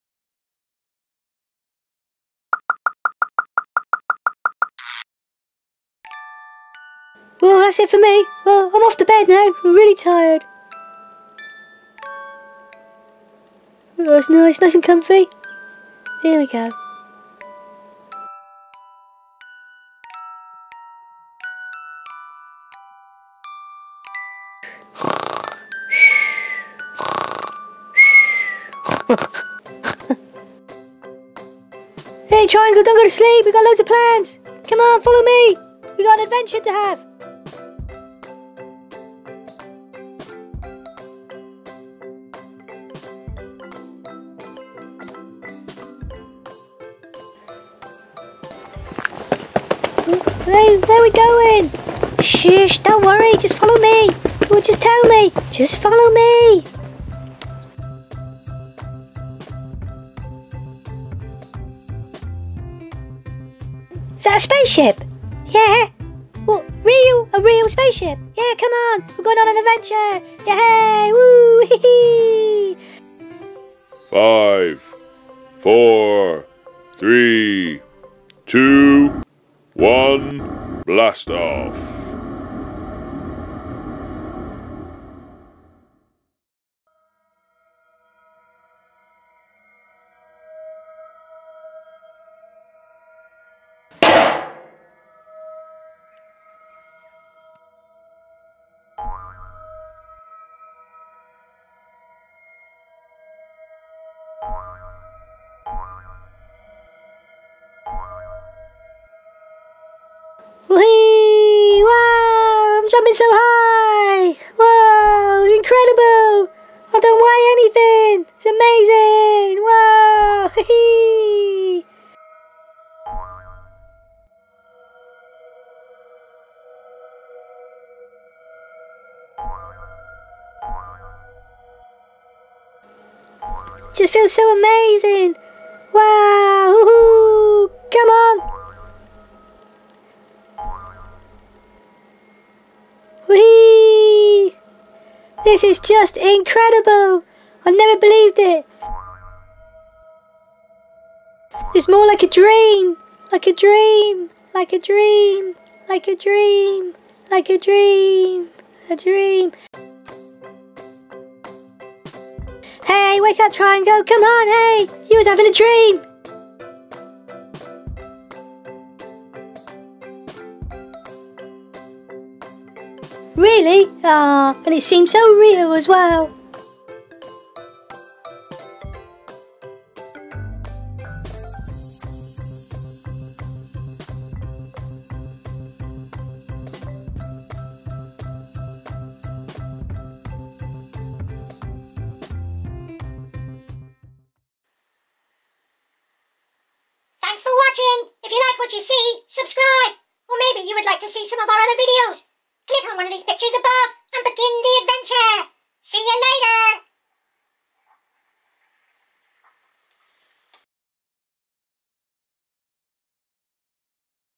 The Shapes | VIVASHAPES | Shapes In Real Life | Moon Bounce | Rocket Trip | Lift Off for kids.
The Shapes ..Shapes in real life..Moon bounce..The shapes in real life are blasting off into space..Come along for the ride.